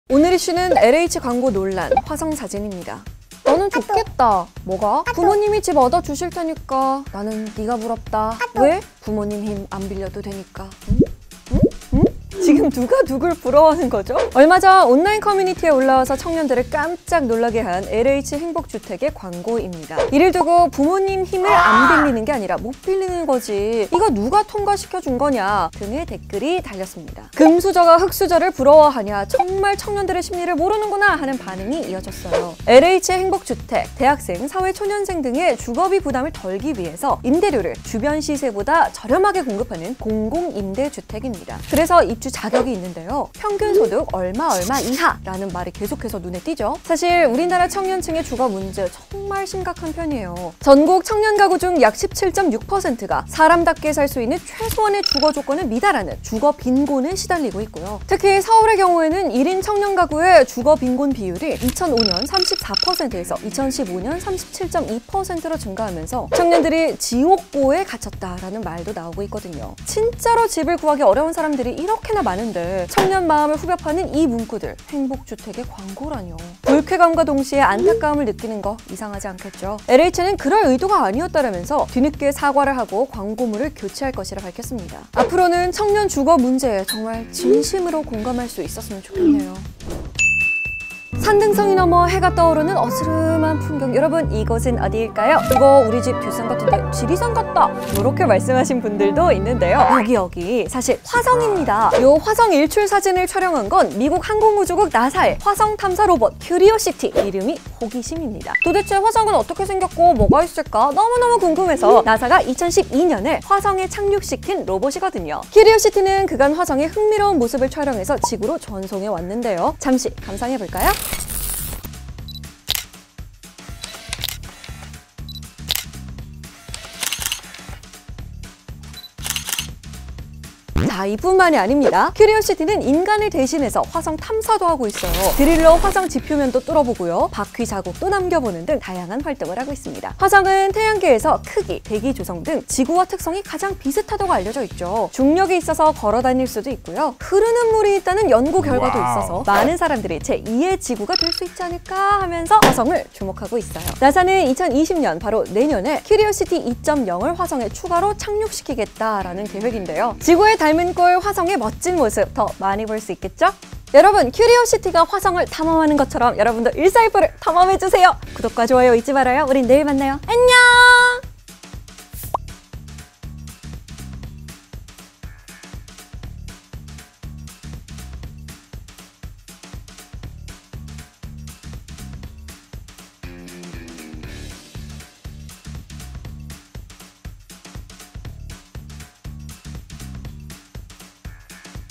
흙수저라 부럽다고? 공감 1도 안 되는 LH 광고 논란 / 14F
LH의 행복주택 광고를 두고 ‘마치 금수저가 흙수저를 부러워하는 것 같다’는 논란이 일었습니다. “청년의 마음에 공감하지 못한다”는 질타가 이어지자, LH는 “그럴 의도가 아니었다”며 광고물을 회수할 것이라 밝혔습니다.